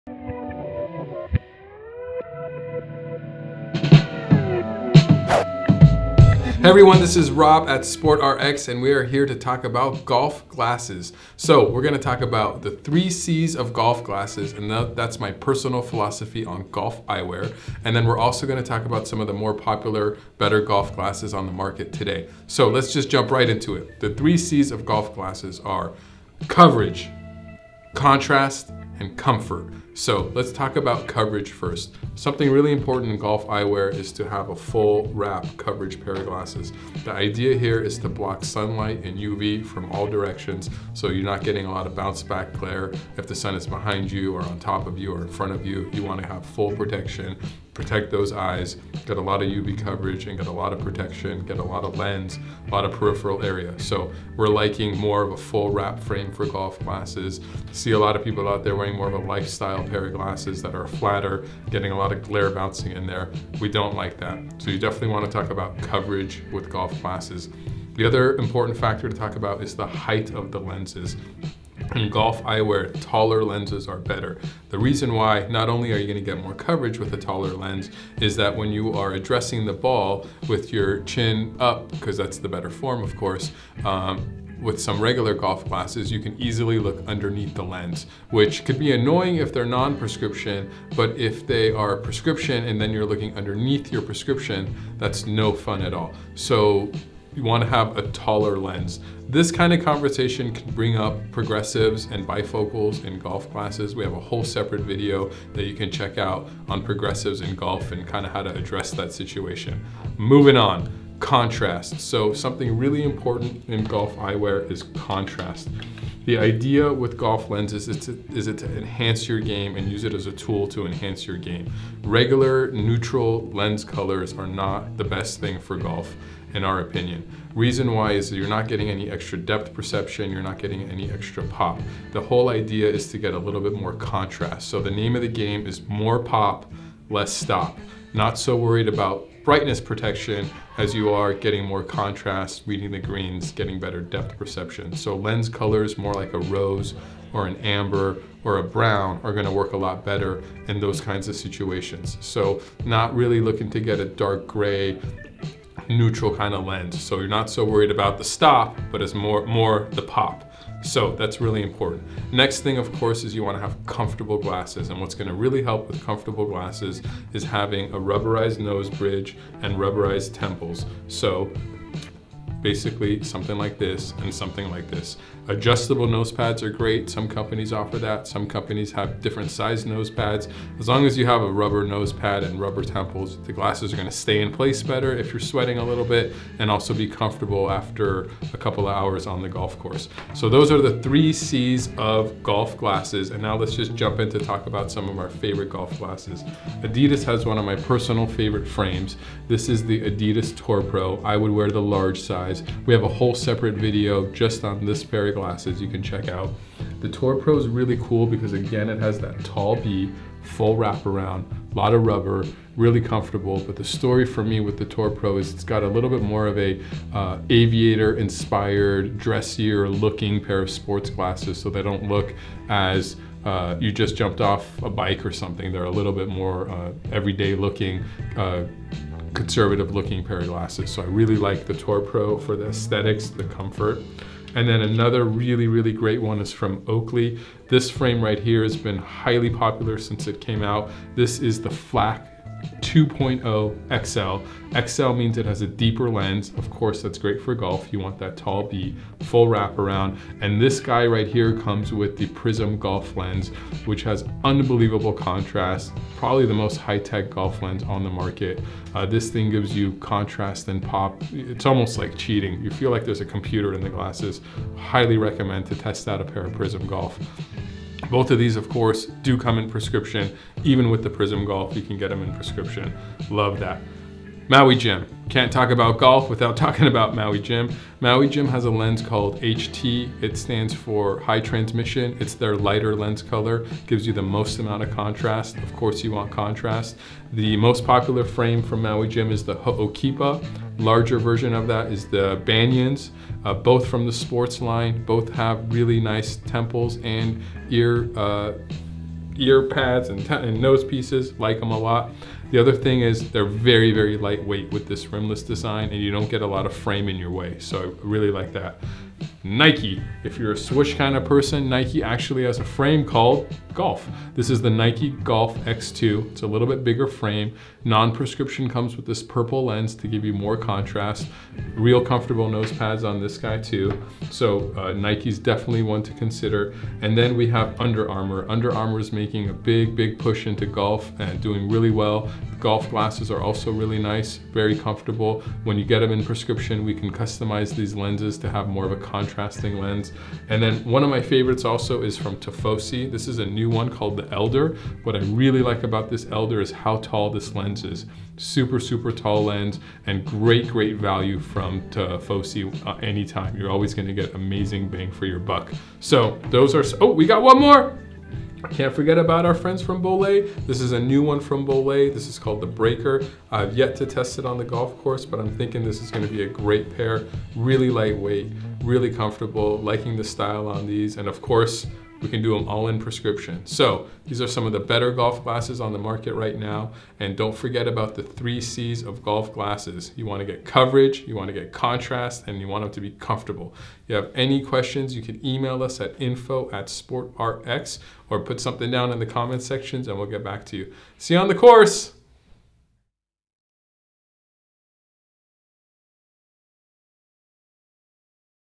Best Golf Sunglasses Guide | SportRx
In the world of golf, there are some key tools that’ll help in lowering your handicap, golf sunglasses being one of those key tools. But not just any ordinary pair of sunglasses that you happen to have in your center console will qualify as the best golf sunglasses. There are actually three essentials to what makes for the best , and we’ve named them the “3 C’s of Golf Sunglasses.” We're here to explain the  three C’s in great detail and highlight some of our favorite frames that exude these three components. You'll soon be on your way to owning the best golf sunglasses out there on the golf course.